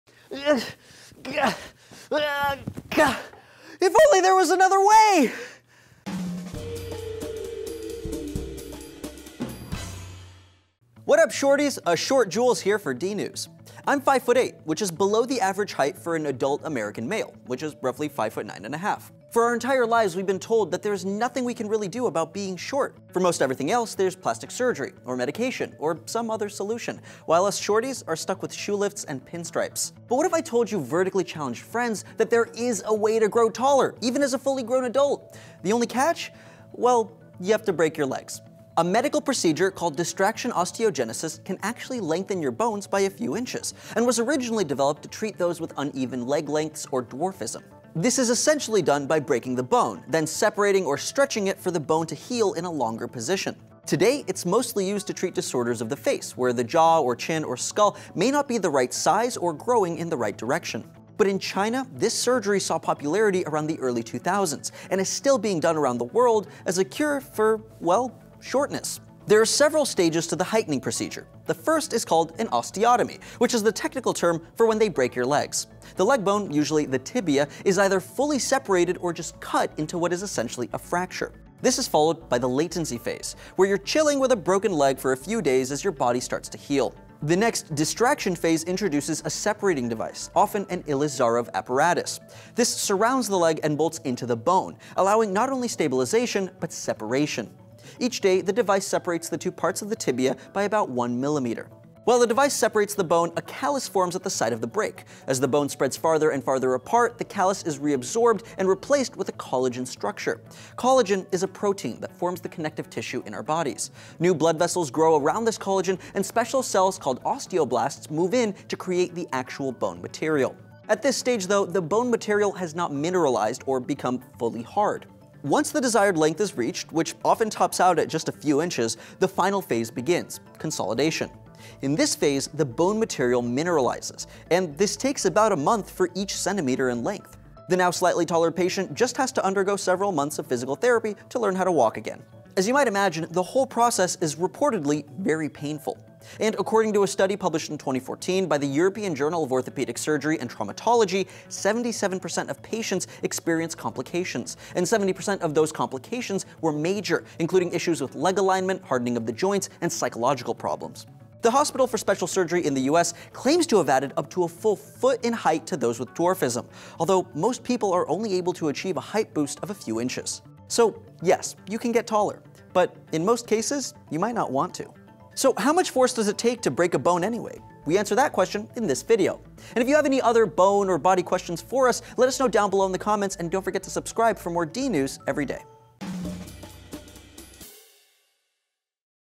This Unorthodox Procedure Makes Short People A Foot Taller
Do you wish you were taller? Well there’s a surgically procedure you could go through to get taller, but it can be pretty painful...

Distraction Osteogenesis for Lengthening of the Tibia in Patients Who Have Limb-Length Discrepancy or Short Stature
“This study was performed to determine the safety and effectiveness of lengthening of the tibia, in patients who have a limb-length discrepancy or a short stature, with use of distraction osteogenesis, a technique based on the principle of distracting the callus that is formed after a subperiosteal osteotomy of the proximal portion of the diaphysis of a long bone.”

Limb lengthening has proven to be a very powerful and effective procedure.
“Limb lengthening is possible and has been performed successfully for about 50 years in Kurgan, Russia. Gavriil A. Ilizarov developed the concept in 1951 after seeing many WWII veterans who had leg fractures that had not healed (non-unions).”

A Closer Look At The Potential Of Bone Lengthening Distraction Osteogenesis
“Bone lengthening techniques have advanced significantly in recent years. Accordingly, these authors discuss the fundamentals of distraction osteogenesis, including surgical techniques, the expression of growth factors and bone morphogenetic proteins, and common complications in an attempt to improve the understanding of the potential of bone lengthening.”

Special thanks to Jules Suzdaltsev for hosting and writing this episode of DNews!